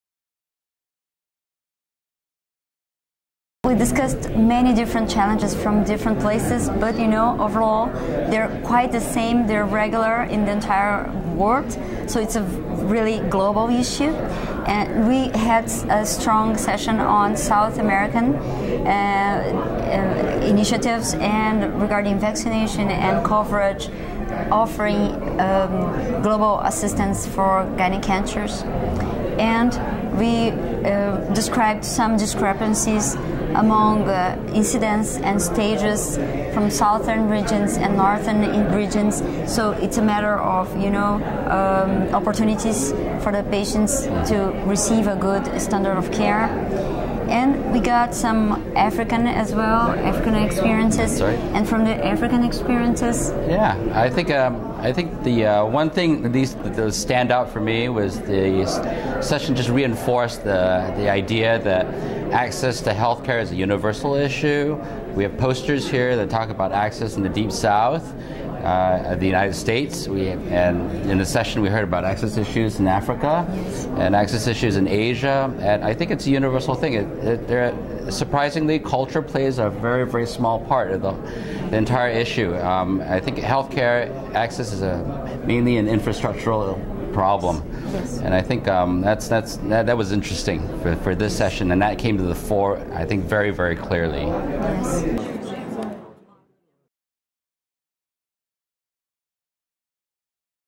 2017 SGO Annual Meeting | Contemporary Issues in Gynecologic Oncology: An International Focus
Course directors Audrey Tsunoda, MD, from the Barretos Cancer Hospital in Brazil and Joseph Soon-Yau Ng, MD, from the National University Hospital of Singapore, had representation from all over the world for their preconference session, "Contemporary Issues in Gynecologic Oncology: An International Focus," and found that access to health care is a "universal issue."